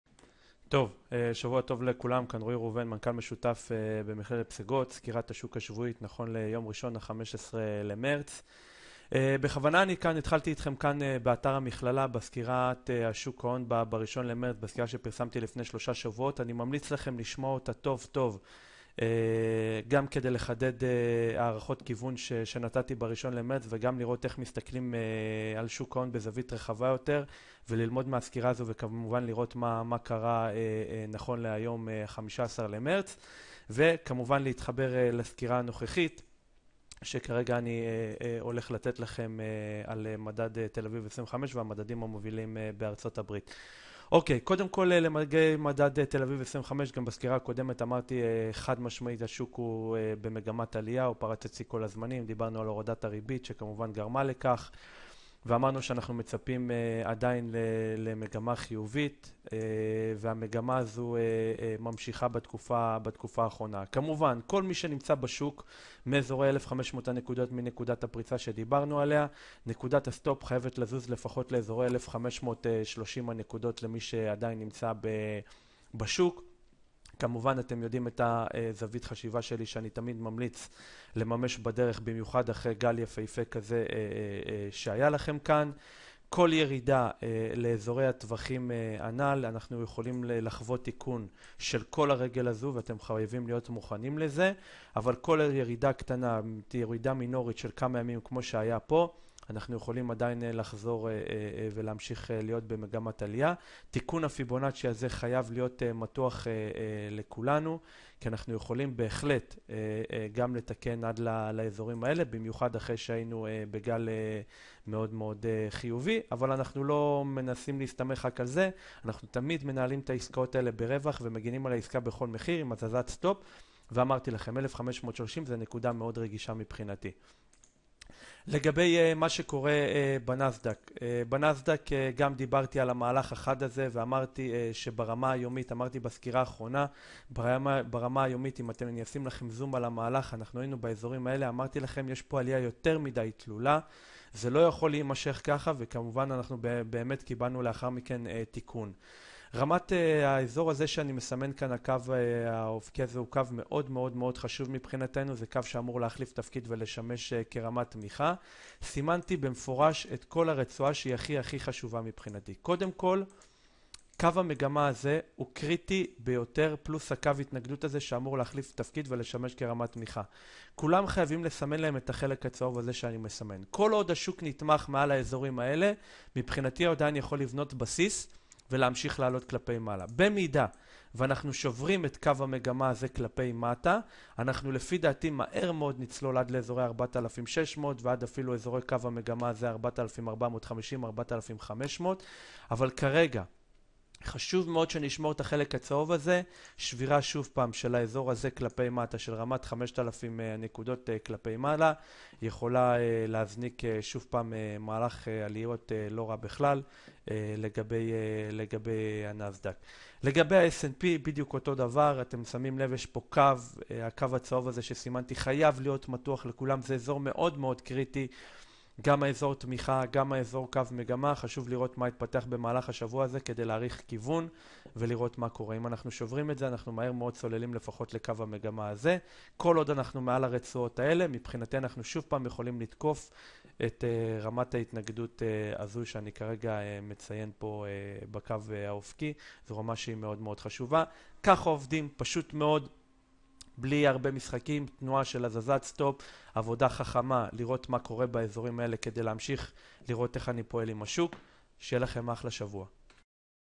סקירה שבועית - מכללת פסגות 15.3.15
להלן סקירת שוק ההון נכונה לתאריך 15/3/2015 - מי שסוקר את מצב השווקים הינו רועי ראובן, מנכ"ל שותף וממקימי מכללת פסגות. היום נדבר על:
- כיוון המדדים המובילים בארץ ובעולם
- נמשיך לדבר על מגמת העלייה מתחילת החודש במדד ת"א 25!
צפייה מהנה!

רוצה לדעת עוד על שוק ההון? כנס לאתר שלנו חל איסור בהסכמים הפרטניים של כל תלמיד על העתקה ו/או שימוש במידע של המכללה.